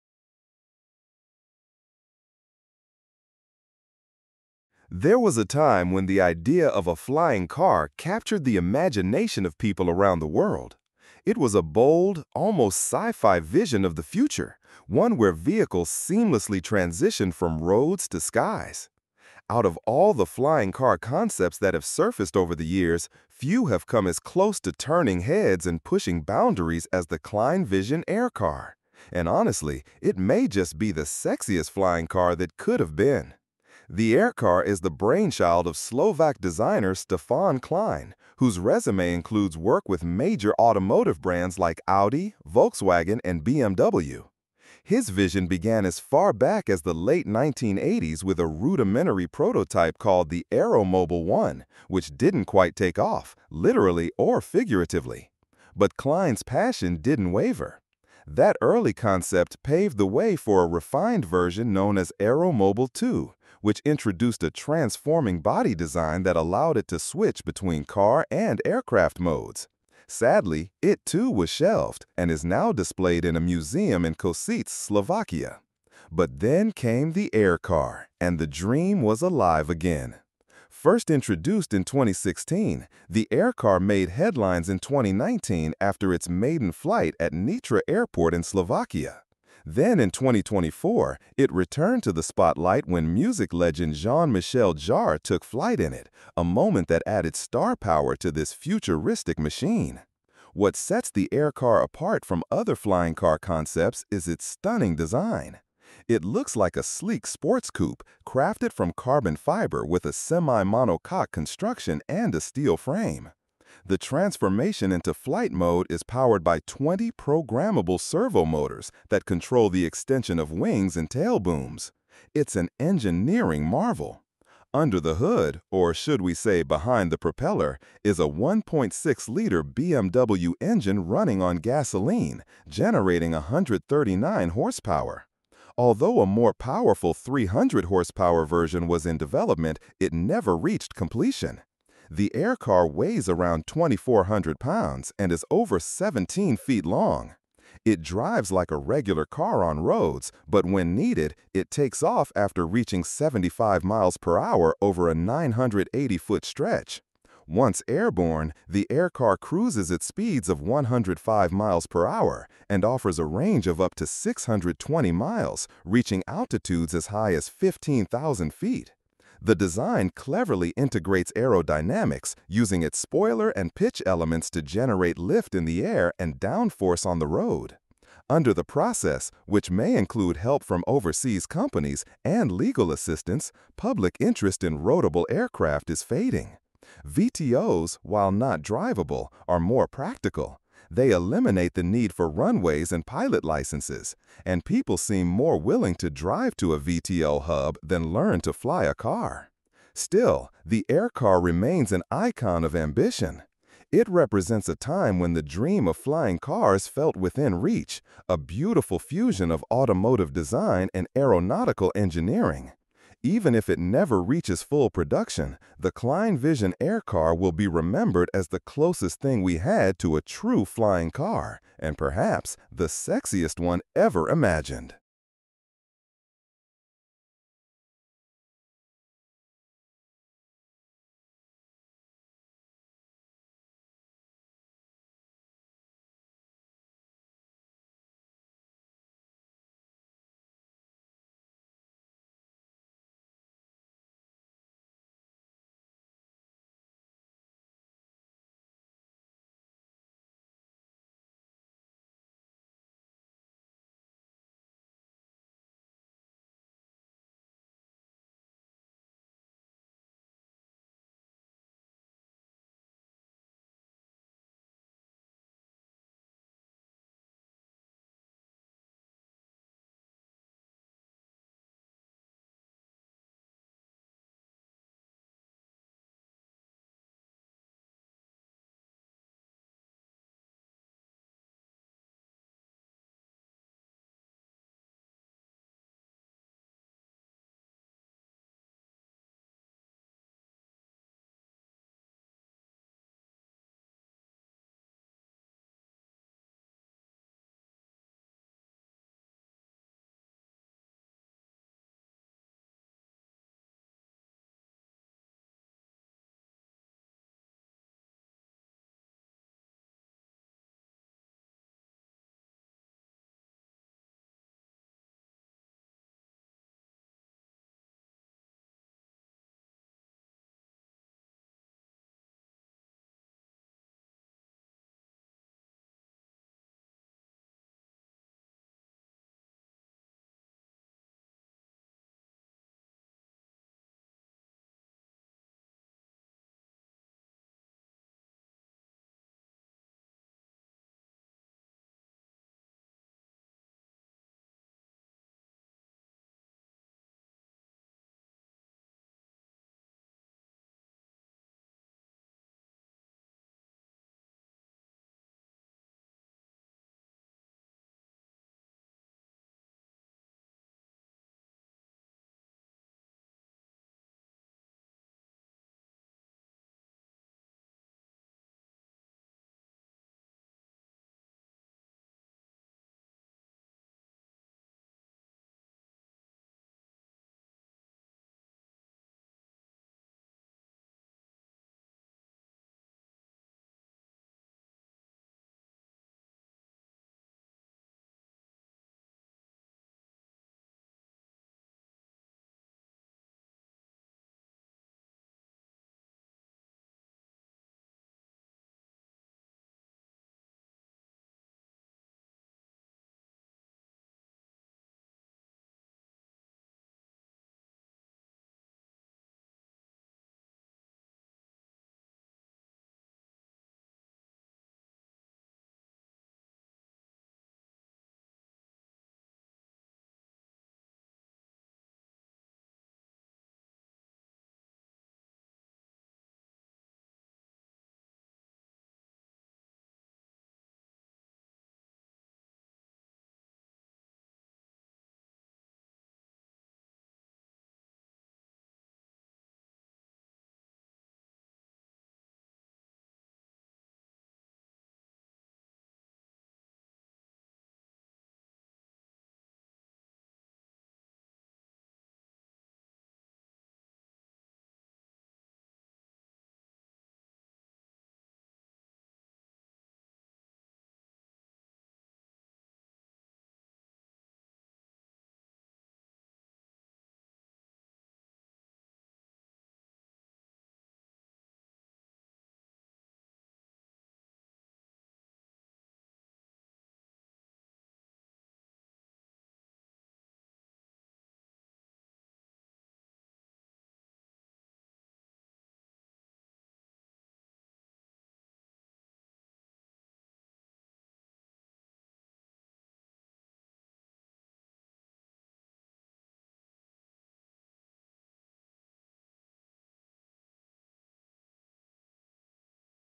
This Is the Sexiest Flying Car That Almost Took Off – KleinVision AirCar Full Story
There was a time when the idea of a flying car captured the imagination of people around the world. It was a bold, almost sci-fi vision of the future—one where vehicles seamlessly transitioned from roads to skies. Out of all the flying car concepts that have surfaced over the years, few have come as close to turning heads and pushing boundaries as the KleinVision AirCar. And honestly, it may just be the sexiest flying car that could have been.

The AirCar is the brainchild of Slovak designer Stefan Klein, whose résumé includes work with major automotive brands like Audi, Volkswagen, and BMW. His vision began as far back as the late 1980s with a rudimentary prototype called the Aeromobil 1, which didn’t quite take off—literally or figuratively. But Klein’s passion didn’t waver. That early concept paved the way for a refined version known as Aeromobil II, which introduced a transforming body design that allowed it to switch between car and aircraft modes. Sadly, it too was shelved and is now displayed in a museum in Kosice, Slovakia.

But then came the AirCar, and the dream was alive again.

First introduced in 2016, the AirCar made headlines in 2019 after its maiden flight at Nitra airport in Slovakia. Then in 2024, it returned to the spotlight when music legend Jean-Michel Jarre took flight in it, a moment that added star power to this futuristic machine.

What sets the AirCar apart from other flying car concepts is its stunning design. It looks like a sleek sports coupe, crafted from carbon fiber with a semi-monocoque construction and a steel frame. The transformation into flight mode is powered by 20 programmable servo motors that control the extension of wings and tail booms. It’s an engineering marvel.

Under the hood, or should we say behind the propeller, is a 1.6-liter BMW engine running on gasoline, generating 139 horsepower. Although a more powerful 300-hp version was in development, it never reached completion. The AirCar weighs around 2,400 pounds and is over 17 feet long. It drives like a regular car on roads, but when needed, it takes off after reaching 75 mph over a 980-foot stretch.

Once airborne, the AirCar cruises at speeds of 105 mph  and offers a range of up to 620 miles , reaching altitudes as high as 15,000 feet . The design cleverly integrates aerodynamics, using its spoiler and pitch elements to generate lift in the air and downforce on the road.

Klein envisioned multiple versions: a two-seater, a three-seater, a four-seater, even a twin-engine and amphibious model. Twelve patents were filed during development, protecting its transformation mechanisms. But unfortunately, that’s where the story starts to lose altitude.

Despite its promise, the AirCar faces a tough reality. Its development has been expensive and time-consuming, and with the world shifting focus to VTOLs (Vertical Take-Off and Landing vehicles), public interest in roadable aircraft is fading. VTOLs, while not drivable, are more practical—they eliminate the need for runways and pilot licenses, and people seem more willing to drive to a VTOL hub than learn to fly a car.

Still, the AirCar remains an icon of ambition. It represents a time when the dream of flying cars felt within reach—a beautiful fusion of automotive design and aeronautical engineering. Even if it never reaches full production, the KleinVision AirCar will be remembered as the closest thing we had to a true flying car, and perhaps, the sexiest one ever imagined.